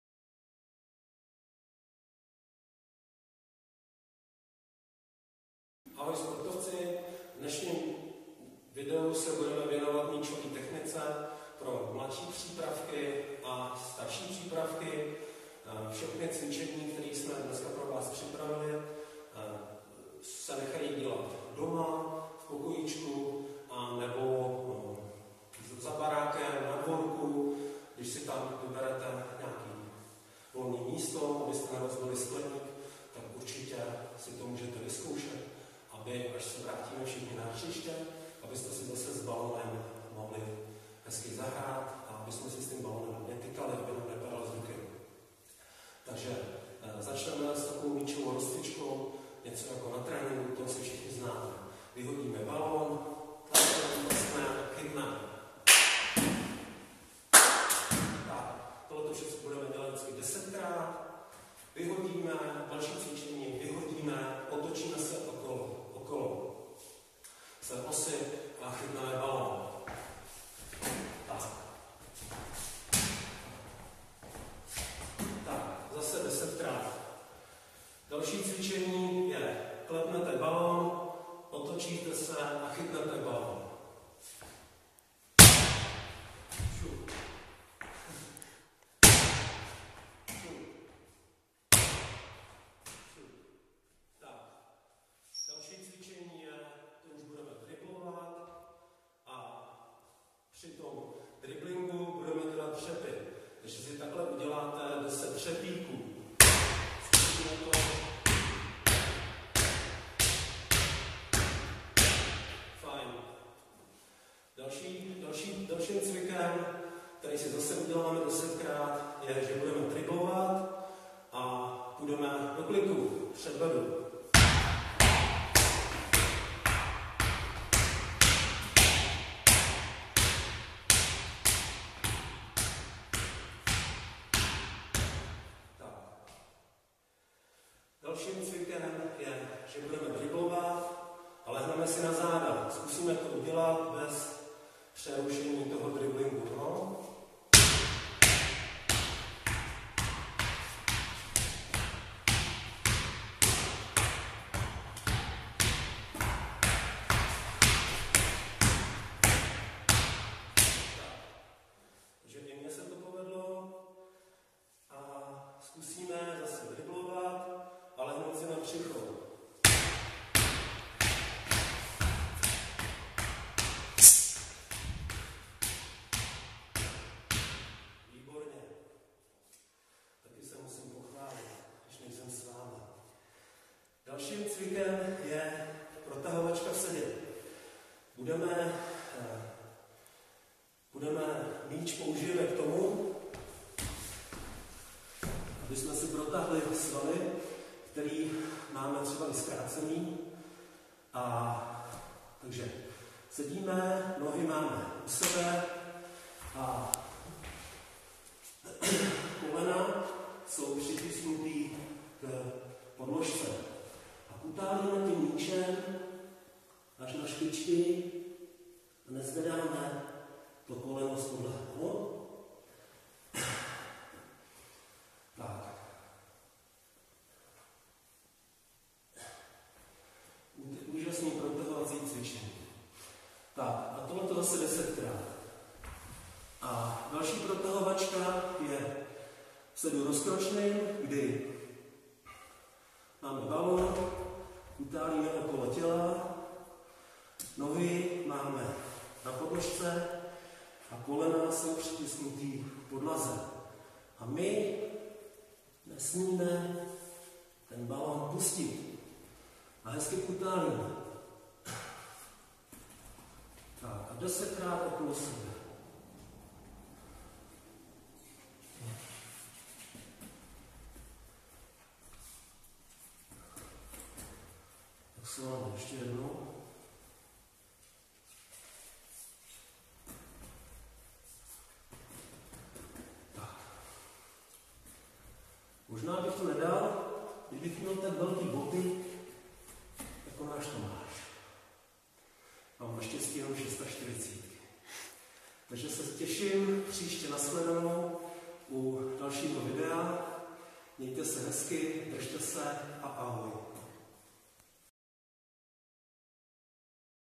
TJ Litohlavy - národní házená. Trénink míčové techniky pro ml a st přípravku
Míčová technika jednotlivce pro mladší a starší přípravku národní házené. Děti si mohou procvičovat dovednosti samostatně doma.